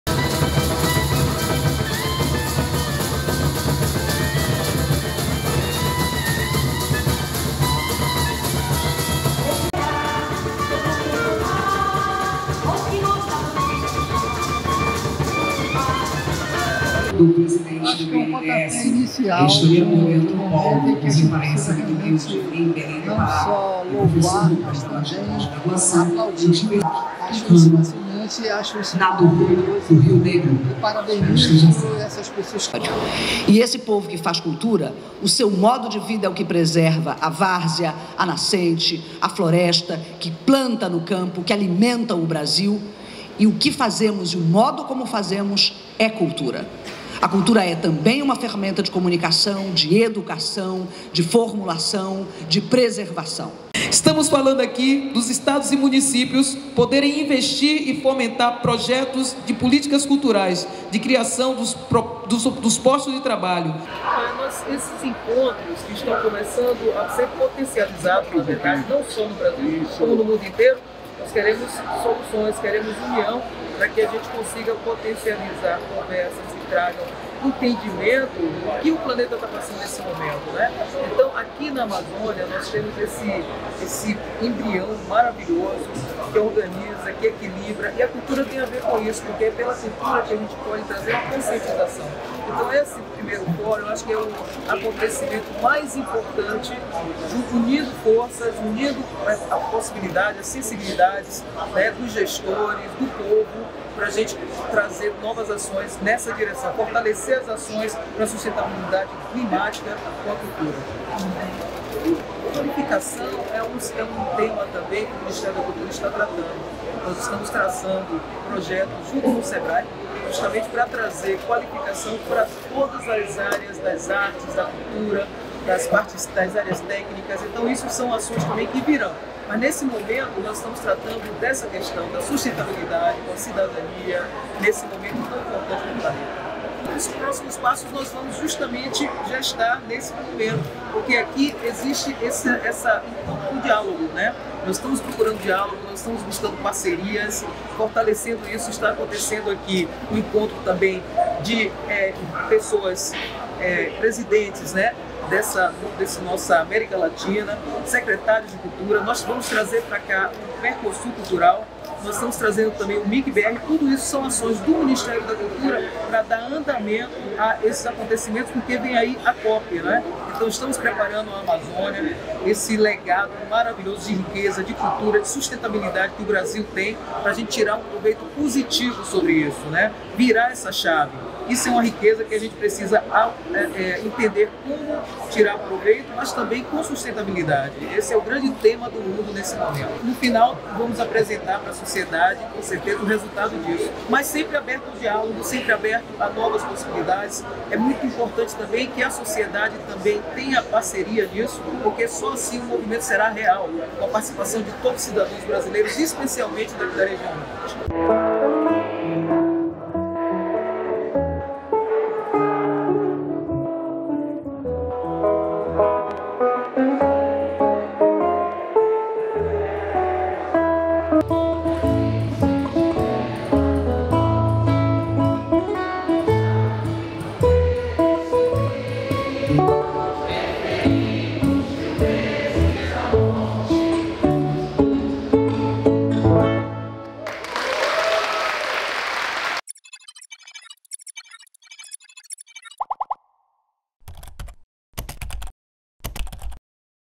Ministra da Cultura Margareth Menezes participa de Fórum Internacional em Belém
Nesta segunda-feira, 7, a ministra da Cultura, Margareth Menezes, participou do I Fórum Internacional de Cultura, Sustentabilidade e Cidadania Climática, que antecede a Cúpula da Amazônia. O evento foi realizado no Teatro Estação Gasômetro, no Parque da Residência, e contou com a presença da secretária de Cultura do Estado, Ursúla Vidal, e outras autoridades.